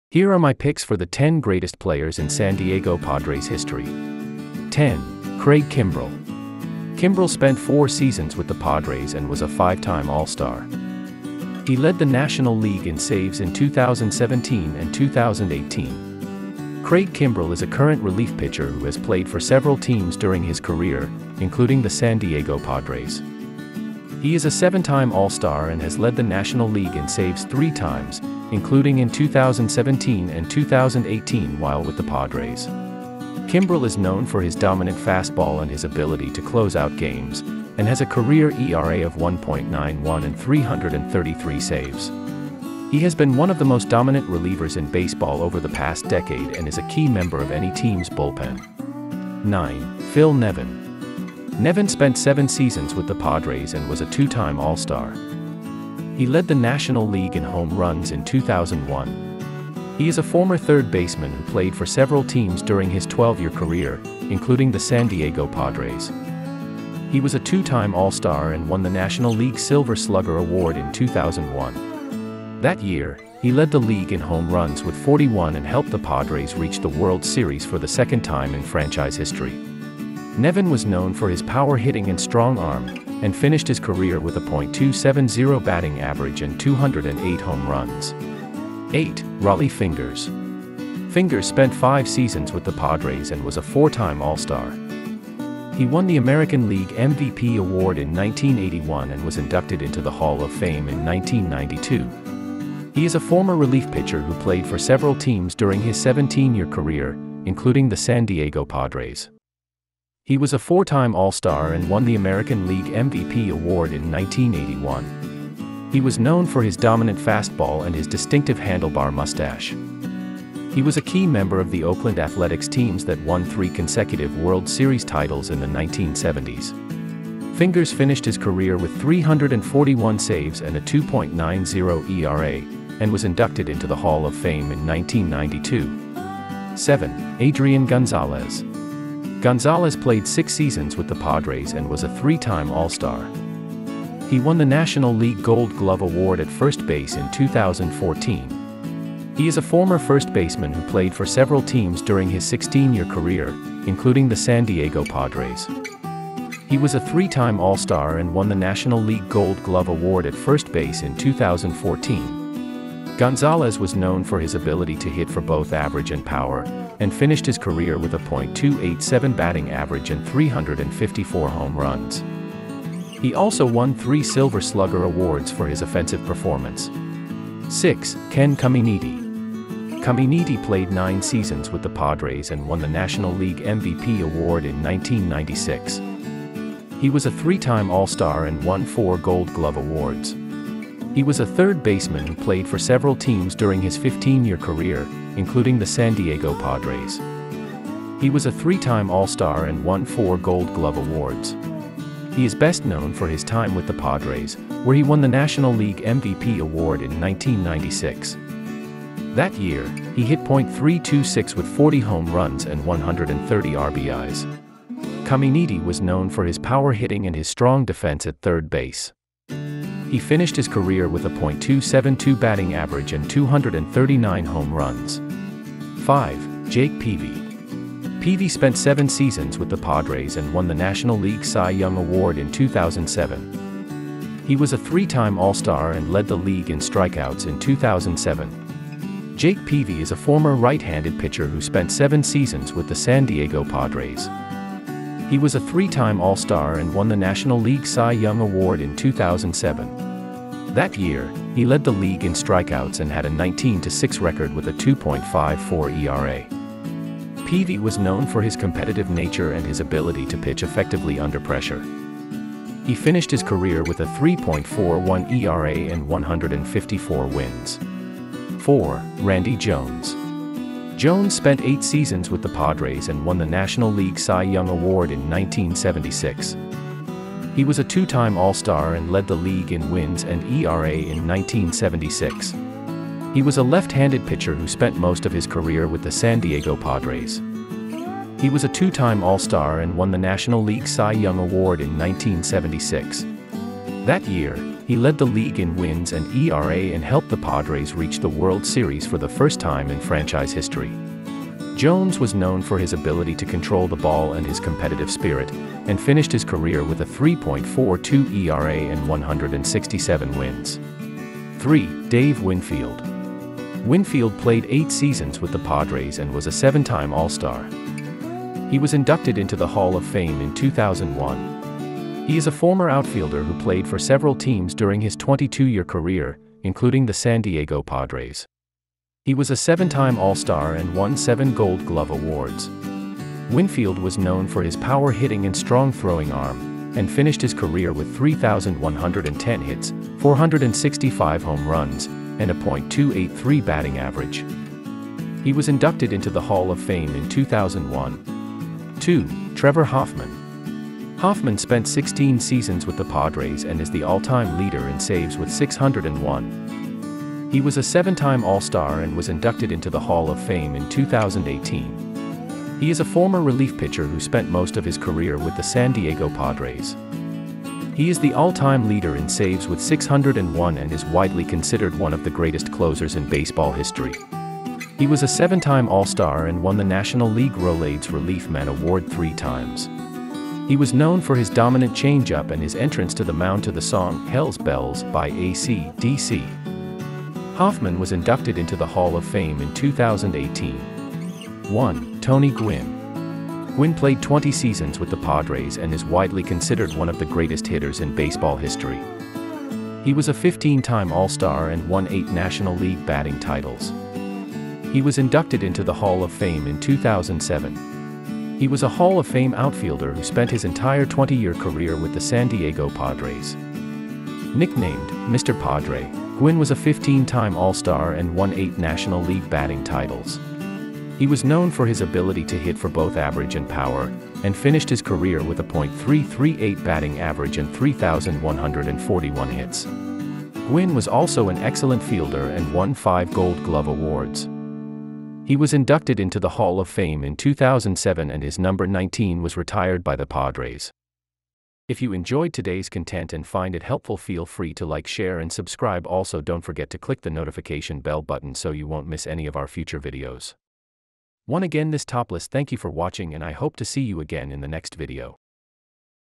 San Diego Padres | The 10 Greatest Players In Of All-time
Today we're going to take a look at the top 10 baseball players of all time on the San Diego Padres.

Our List:
00:00 10. Craig Kimbrel
00:49 09. Phil Nevin
01:29 08. Rollie Fingers
02:16 07. Adrian Gonzalez
02:57 06. Ken Caminiti
03:45 05. Jake Peavy
04:28 04. Randy Jones
05:11 03. Dave Winfield
05:48 02. Trevor Hoffman
06:31 01. Tony Gwynn